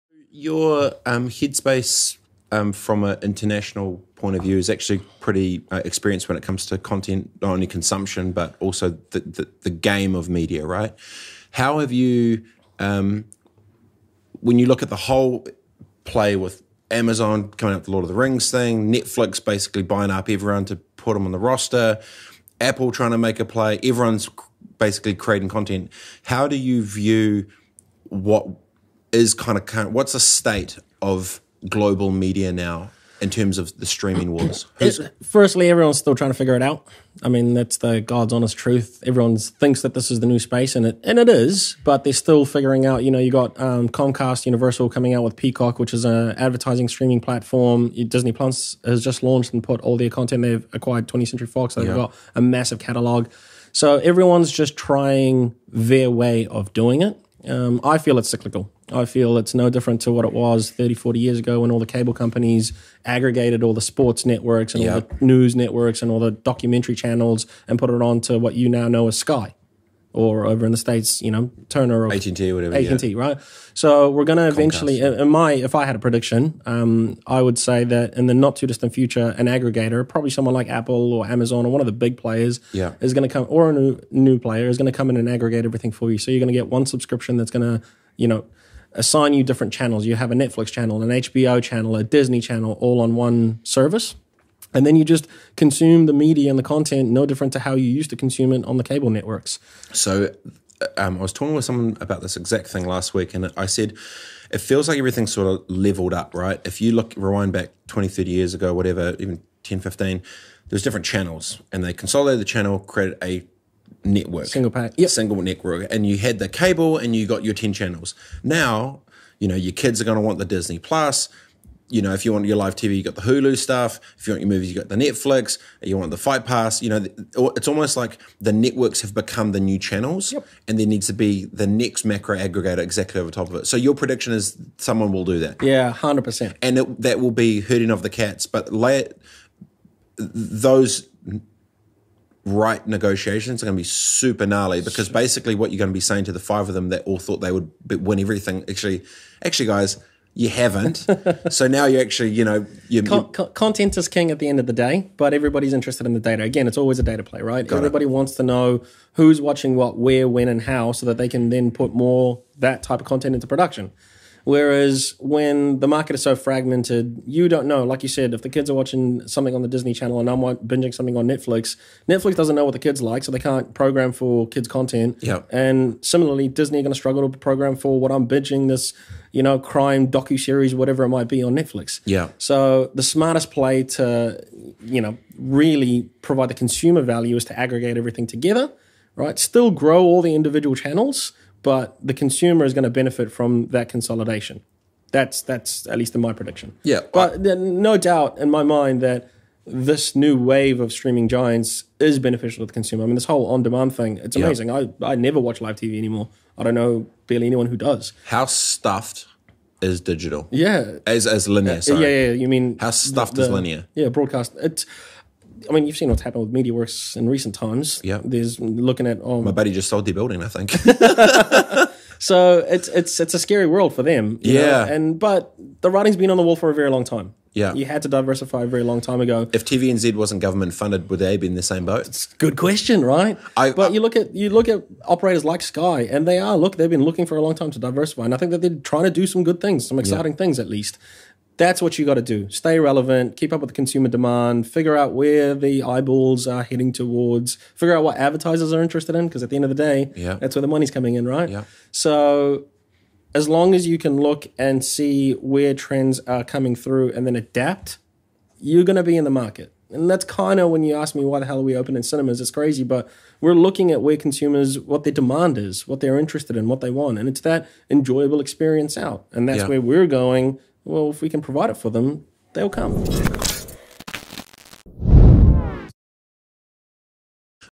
The State of Global Media w/ Managing Director Of The Otter Group: Ahmed Almuhkter
Listen to the full interview over on the SWITCH Aotearoa Podcast on Spotify: